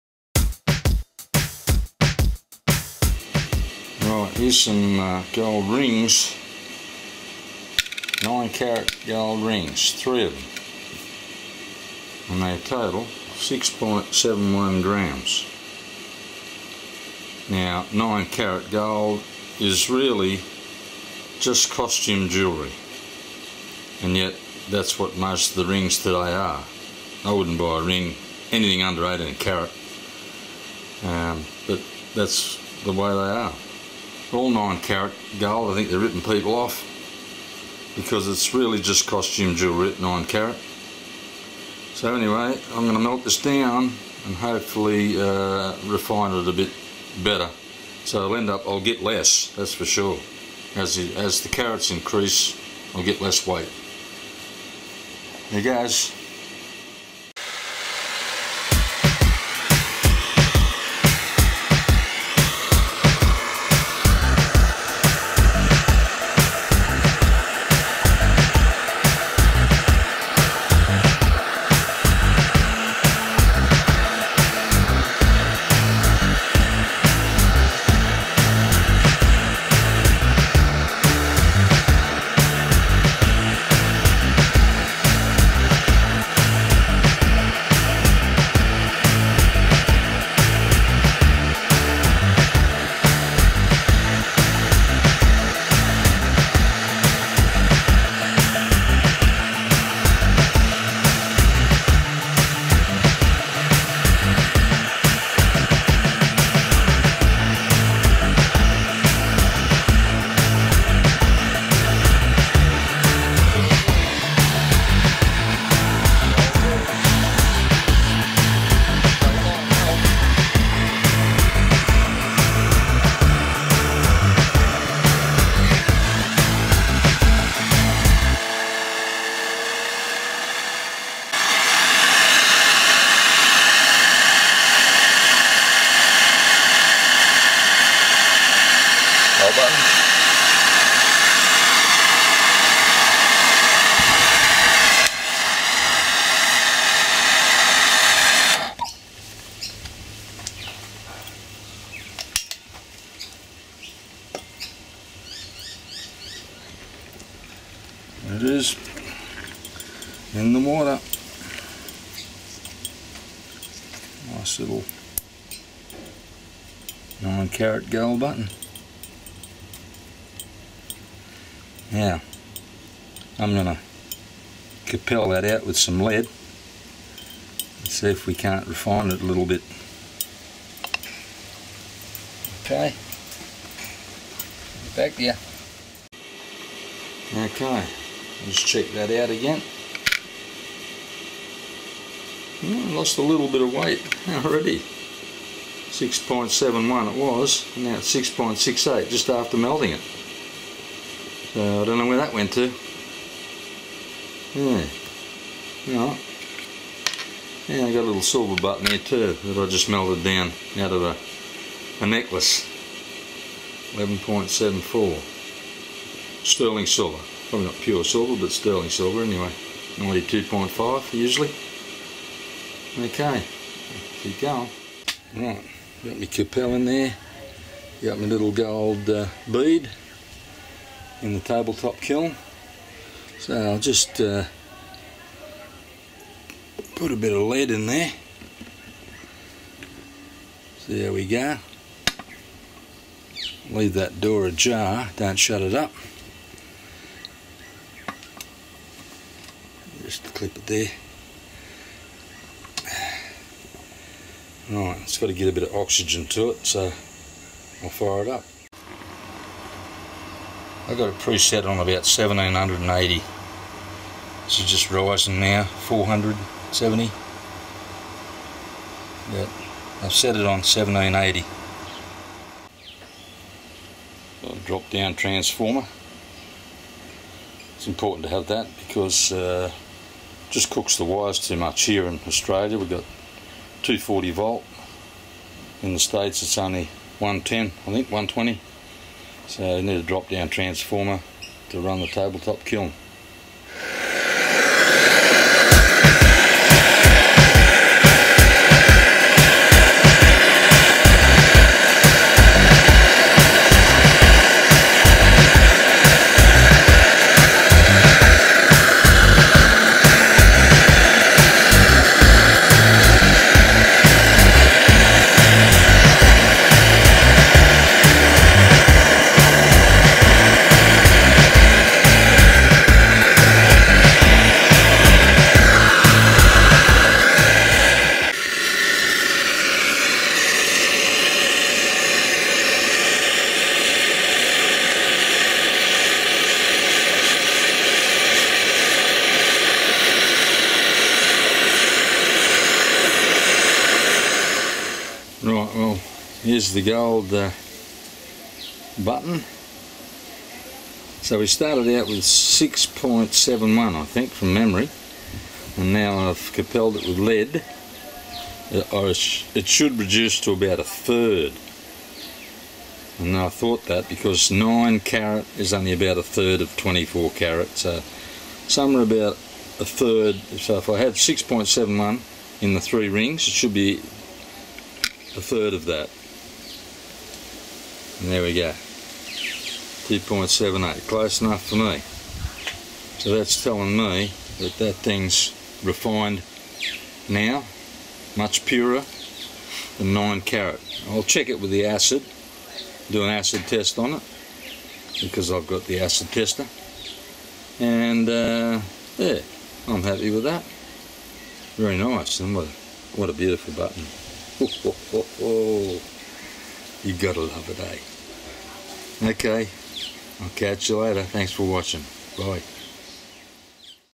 Refining 9 Carat Gold costume Jewelry # Gold Hunter #
I set out to refine some 9 carat costume jewelry to a higher carat yield by melting and cupelling with lead rather than acids . Now because 9 carat is only about a 3rd of 24 carat then my initial weight should drop by about two  thirds so after a few hickups with my Table top kiln i had to finish the job manually with Map gas , but still got a result i was happy with .